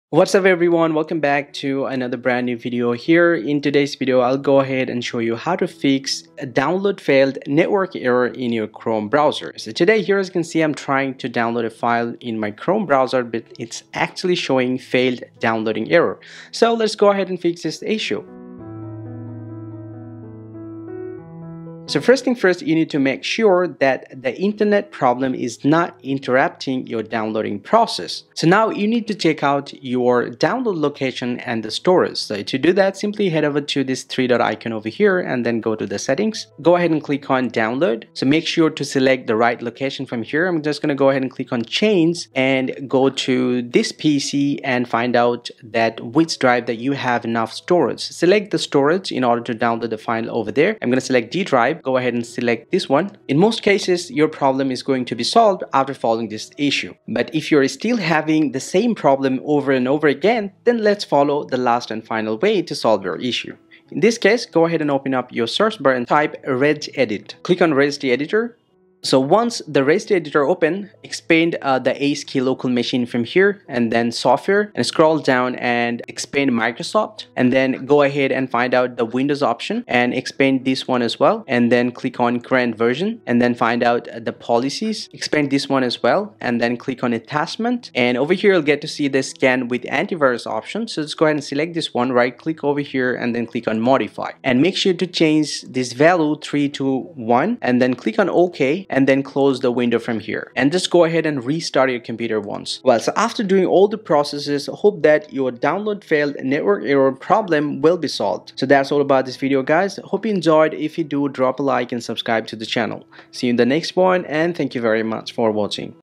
How To Fix Google Chrome Download Failed! [Network Error]
Trying to download a photo/ video/ Audio/ Doc File from your Google Chrome Browser but can’t? it's showing Failed- Download error or Network failed Error? The 3 quick and easy solutions to fix Google Chrome download failed error on Windows 10/11 computers.

0:00 Google Chrome Download Failed Problem
0:21 Solution 1: Check the Internet
0:32 Solution 2: Check and Change Download Location
1:03 Solution 3: Modify Registry Editor Setting